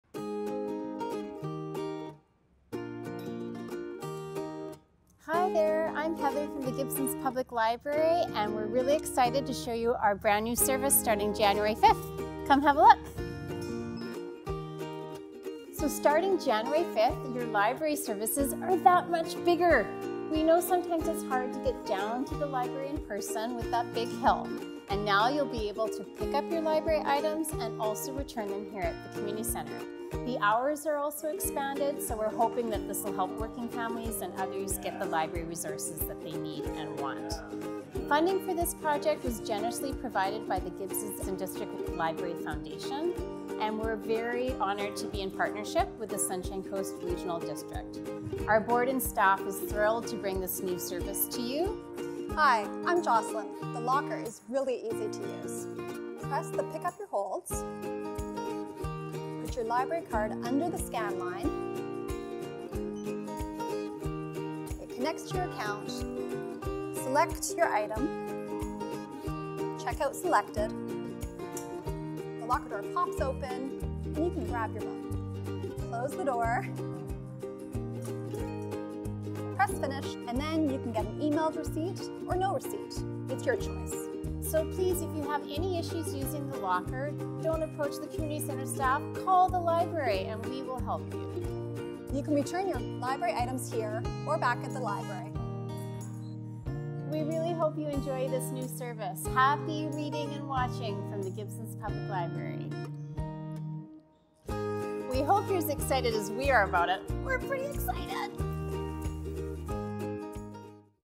Library Locker
Introducing. the Library Locker!
You can now pick up your library holds and return your items at our new Library Locker at the Gibsons & Area Community Centre! See how easy it is to use.